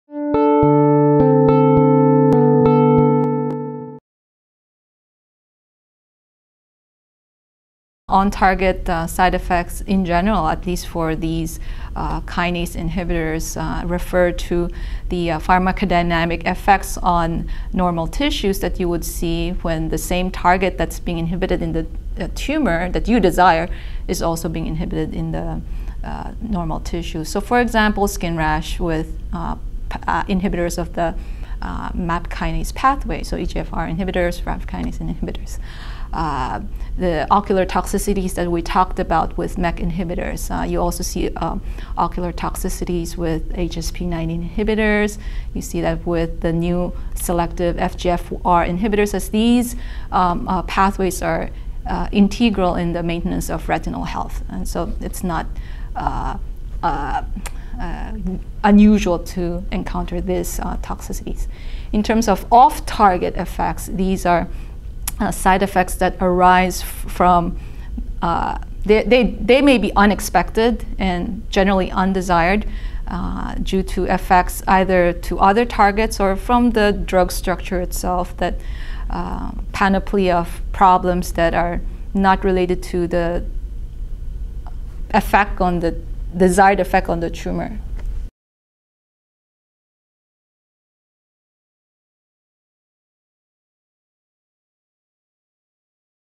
Dr. Dy Discusses On-Target and Off-Target Side Effects
Grace Dy, MD, Associate Professor, Department of Medicine, Roswell Park Cancer Institute, gives an overview of on-target and off-target side effects from anticancer targeted agents.